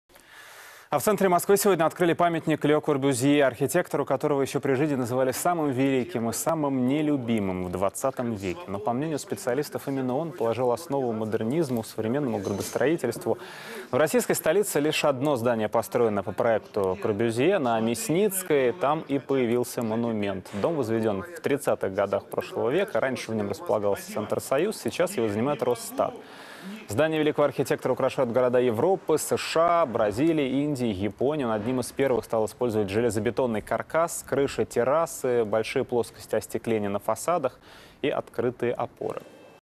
В центре Москвы открыли памятник Лё Корбюзье
Архитектора ещё при жизни называли самым великим и самым нелюбимым в XX веке. Но, по мнению специалистов, именно он положил основу модернизму и современному градостроительству.